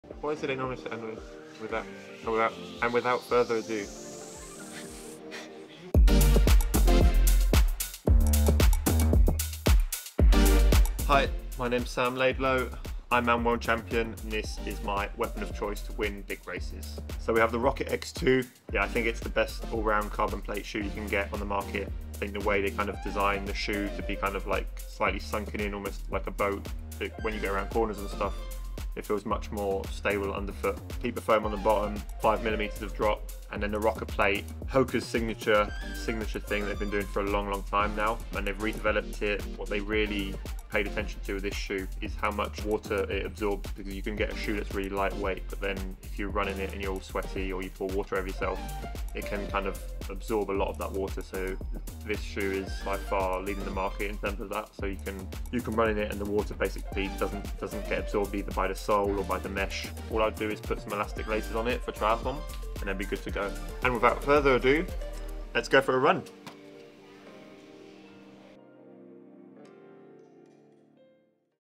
Sam Laidlow on the Rocket X 2 🚀
At this weekend's VinFast IRONMAN World Championships in Kona, @samlaidlow will look to defend his World Championship title in the Rocket X 2. Here’s why the champ calls it, “the best all-around carbon shoe you can get.”